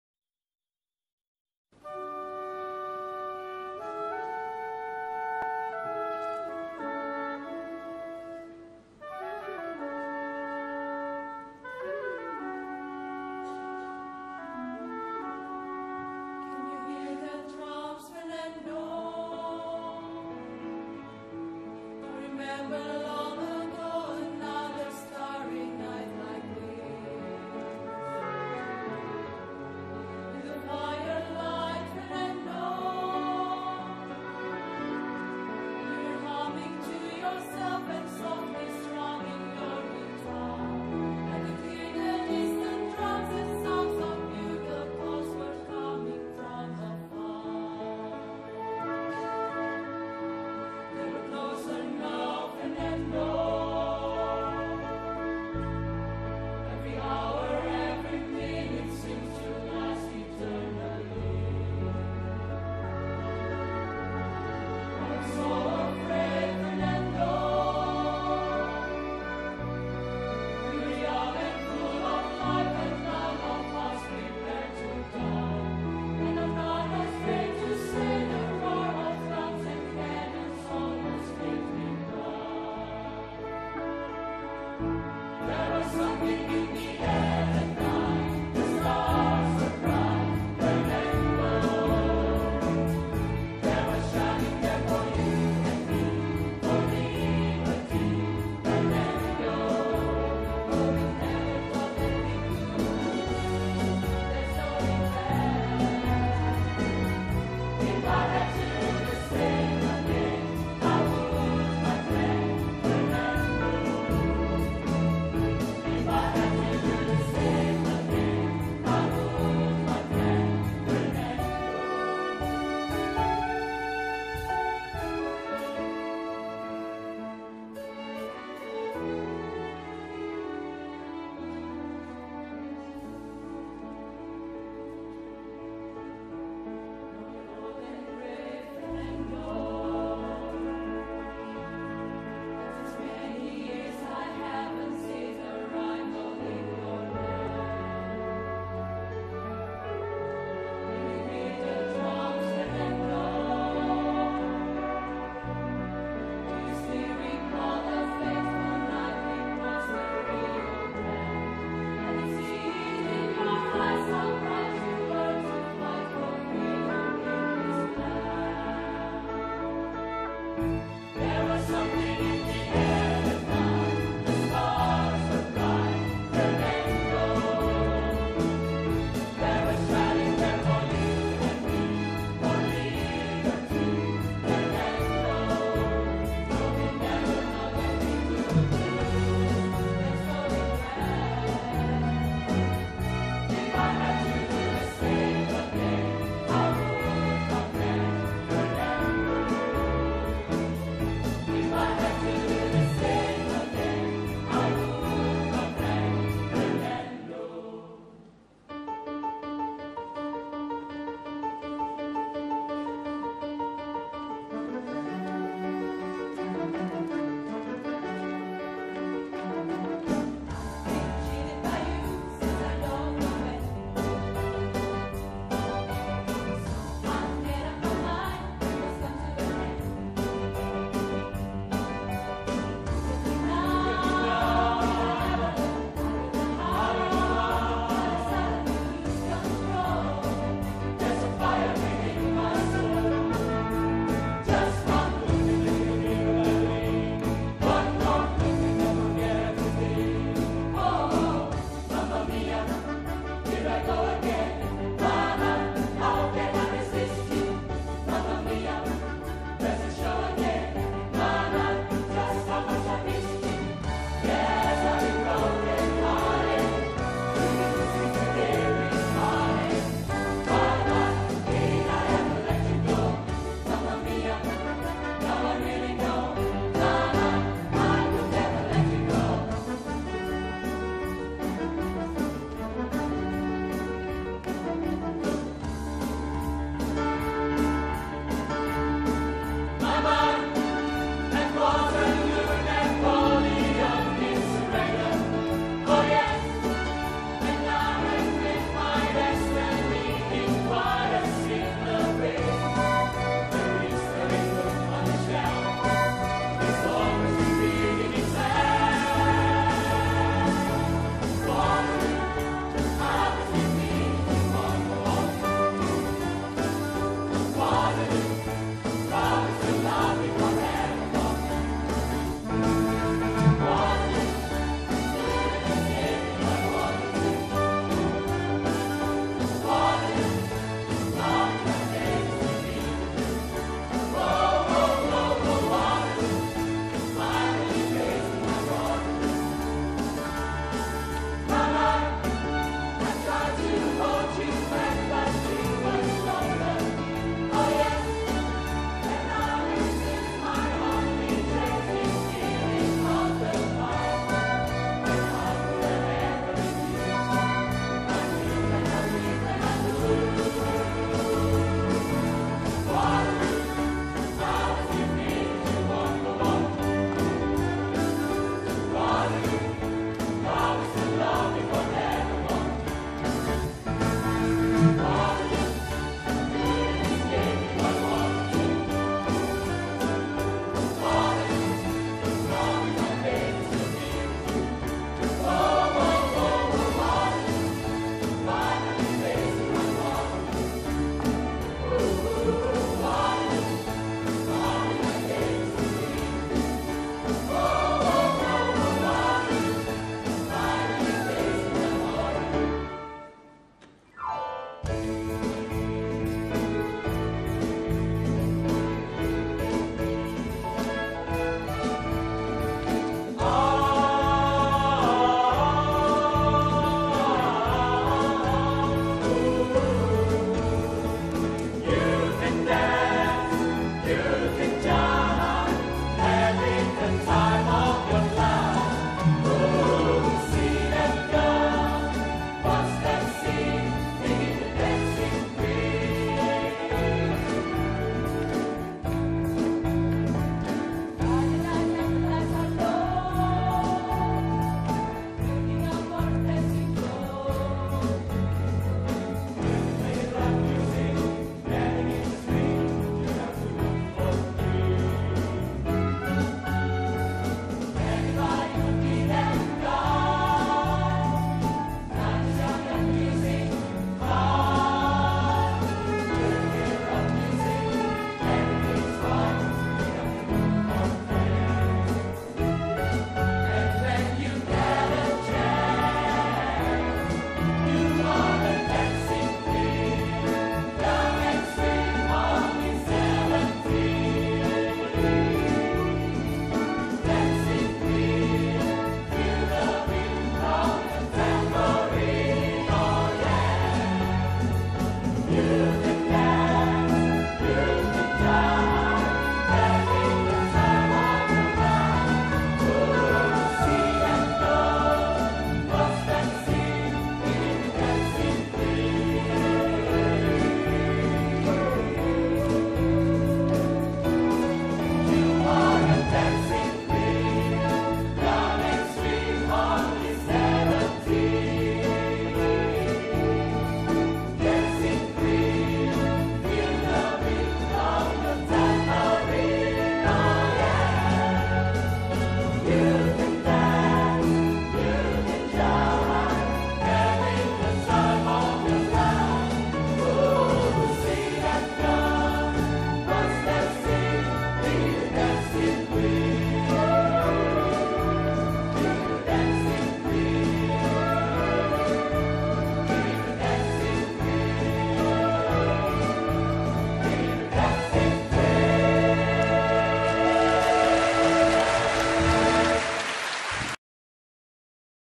Eliata Choir - ABBA Medley
Can't wait for uploading this one. It is really good!! Not because I was involved in the production but when I was objectively listening to it with headphones, seemed it was not recorded from a live show. Everything's so perfect, not like choir singing with moving, the music also like production from studio. So I directly uploaded this to my channel. Congratulations to Maya Widiyaningrum who is very succesful in coaching and building this awesome choir..!!
Live recorded, mixed and mastered by Erwin J. Krisnanda